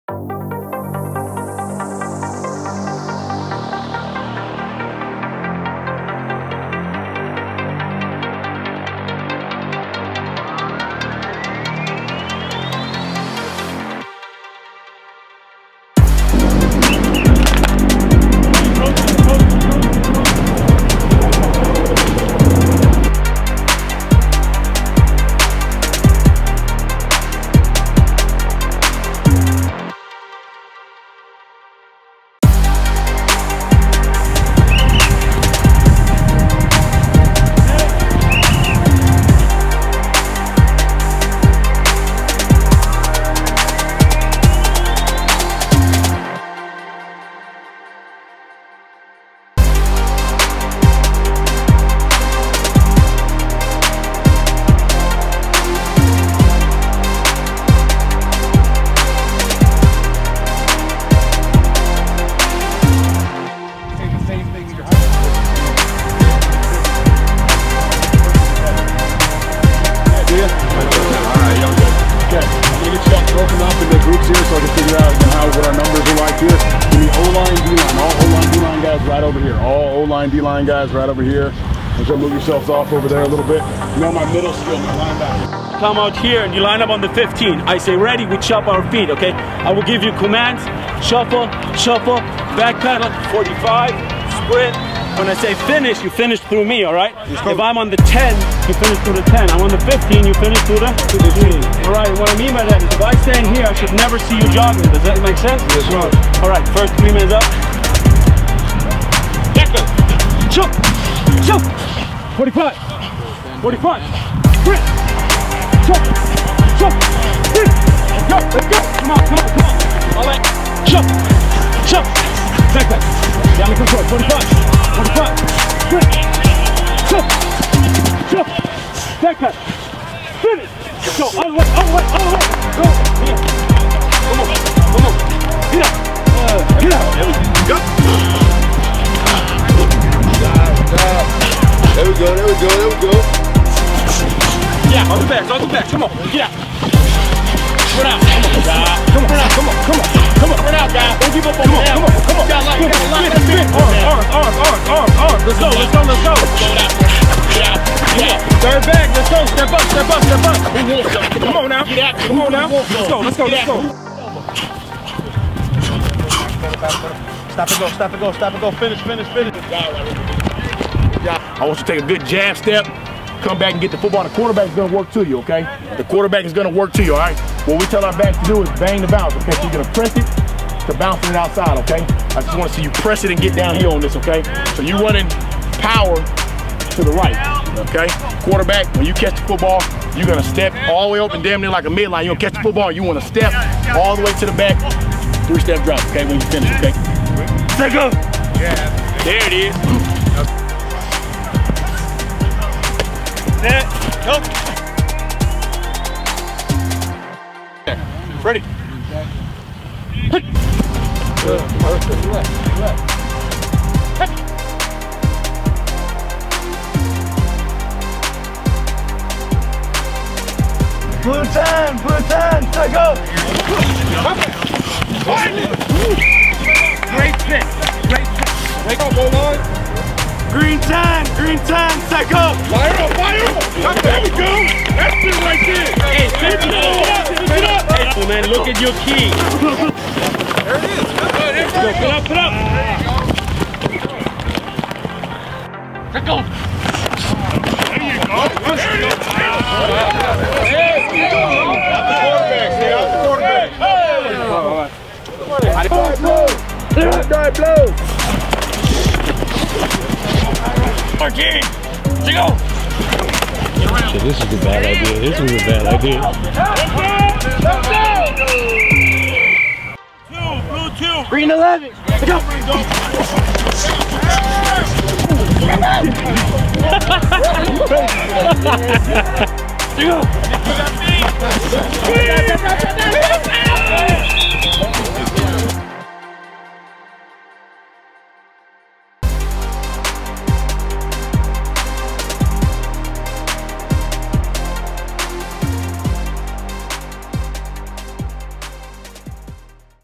2016 WNMU padded camp highlights
Highlights from the 2016 WNMU padded camp at West Hills HS. Featuring coaches from Western New Mexico University, Weber St, Dixie State,  Azusa Pacific, Hastings College, Whittier College, and Trinity International.